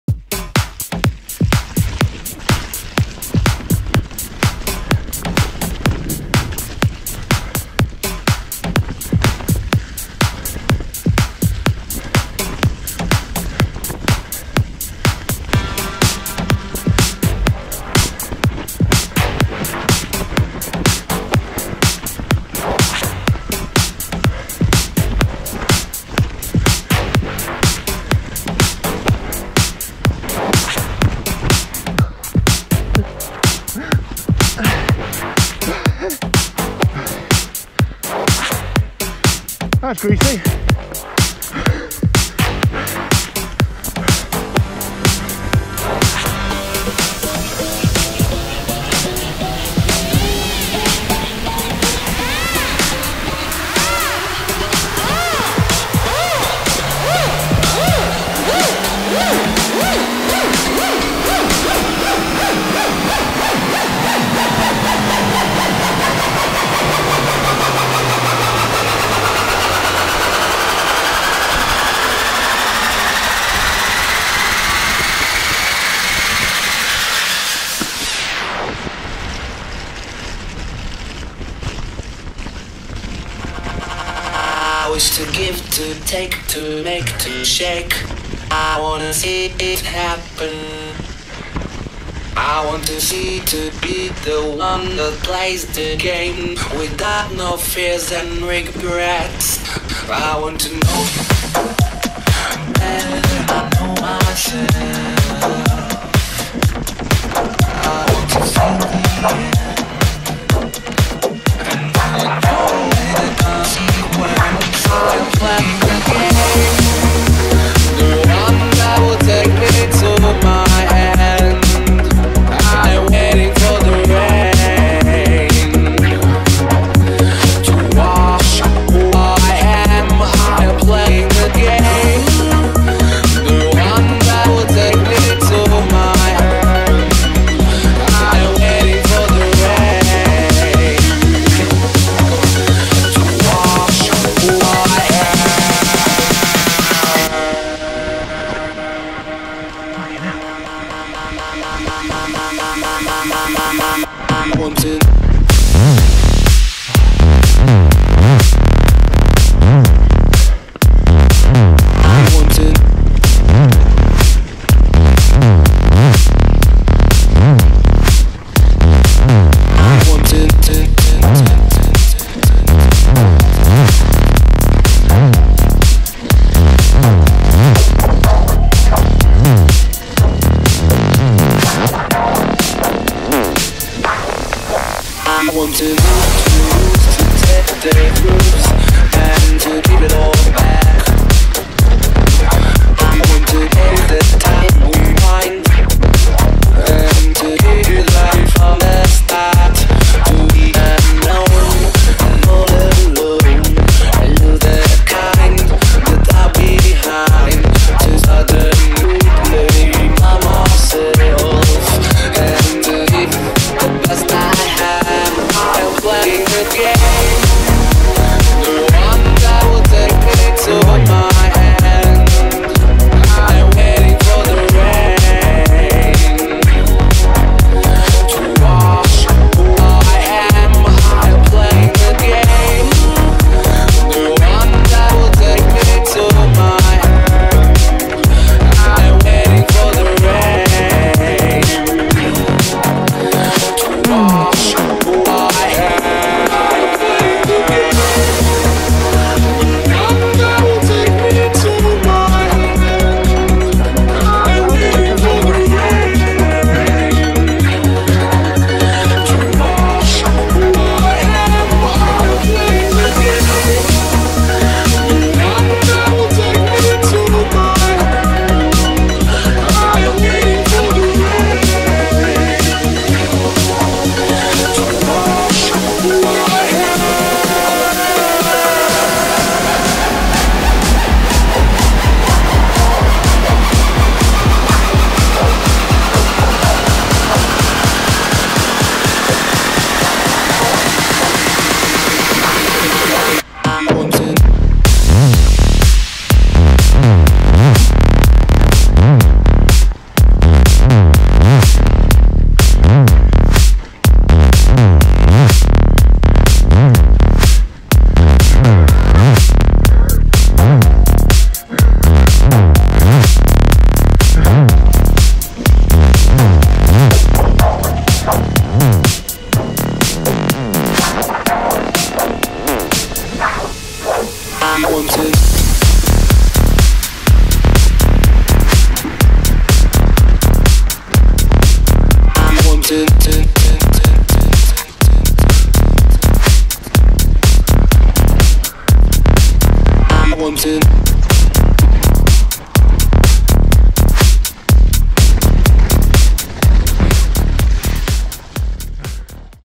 Practice runs FOD
clips of the Haibike mini Enduro at Forest of Dean on march 8th
this was extremely greasy but epic fun
Recorded on chest mounted gopro hero 8
riding 2020 Trek remedy 8 xt